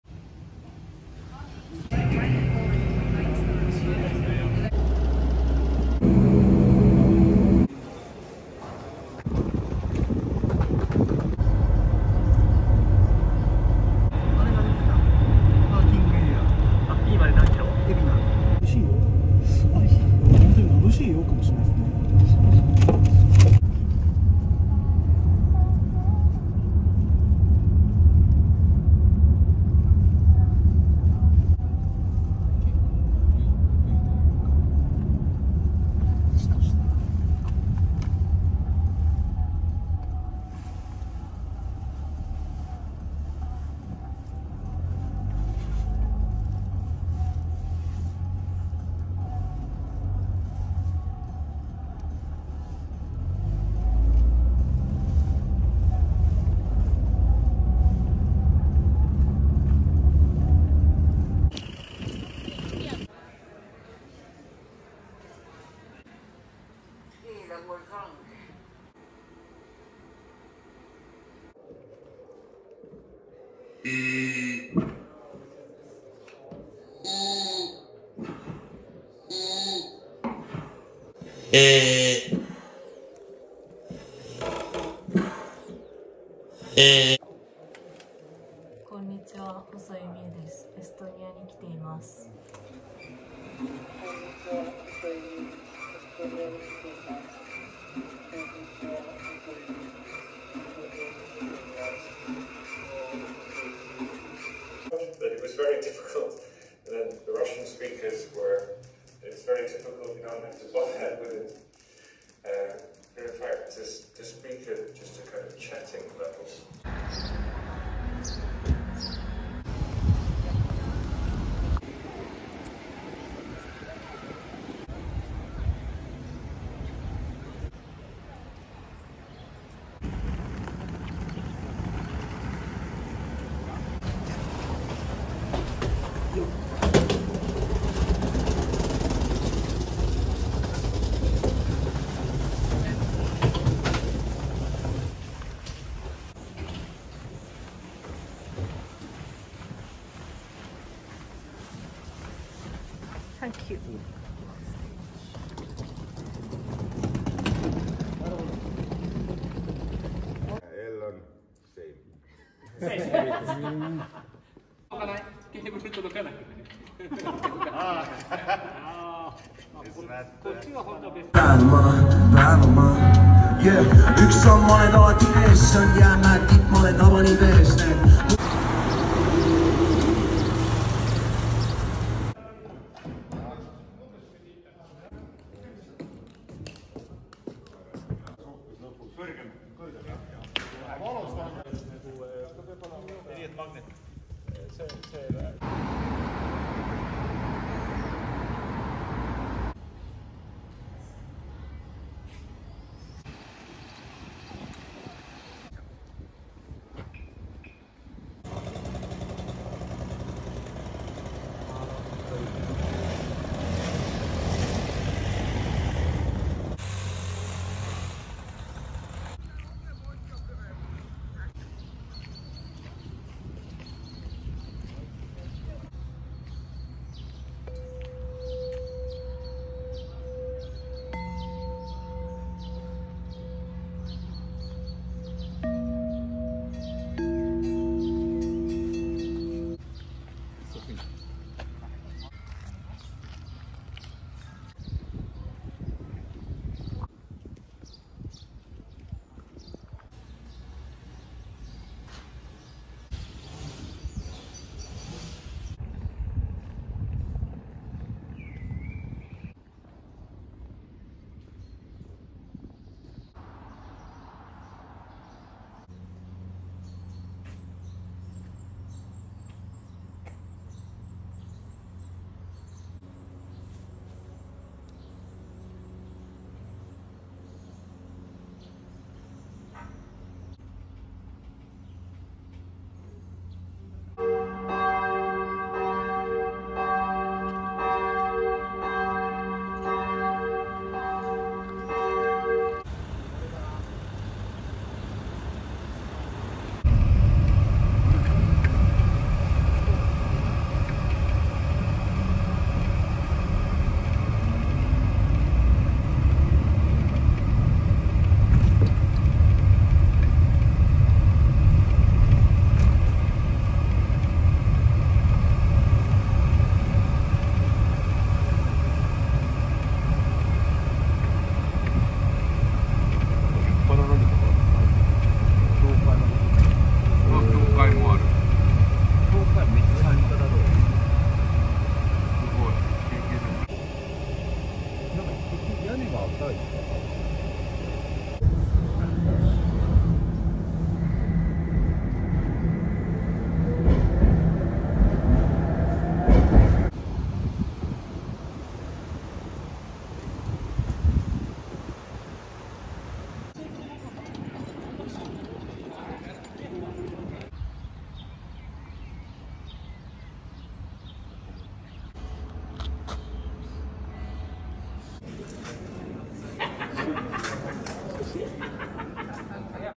Estonia 2024
Estonian National Museum, Teater Vanemuine, University of Tartu Museum, Ruins of Tartu Cathedral, Tartu Artists Union, Old town of Tallinn

Estonia visit for Shiro Takatani "Tangent" European Premier
European Capital of Culture Tartu 2024
June 4,5th @Teater Vanemuine